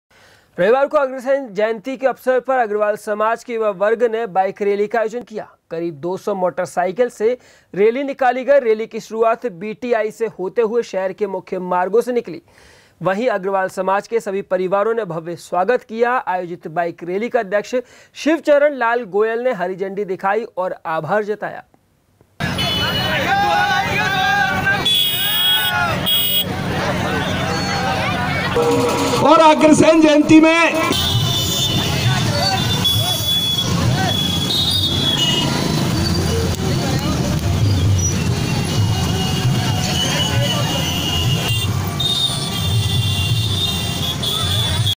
sabalgarh बाइक रैली
रविवार को अग्रसेन जयंती के अवसर पर अग्रवाल समाज के युवा वर्ग ने बाइक रैली का आयोजन किया। करीब 200 मोटरसाइकिल से रैली निकाली गई।  रैली की शुरुआत बीटी आई से होते हुए शहर के मुख्य मार्गों से निकली। वहीं  अग्रवाल समाज के सभी परिवारों ने भव्य स्वागत किया। आयोजित बाइक रैली का अध्यक्ष शिवचरण लाल गोयल ने हरि झंडी दिखाई और आभार जताया।